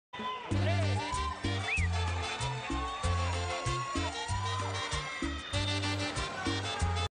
Fiebre tricolor I
Los turistas que están de paso en Barranquilla, se dejan contagiar de la 'fiebre amarilla' que provoca la selección Colombia, de cara al encuentro contra Venezuela.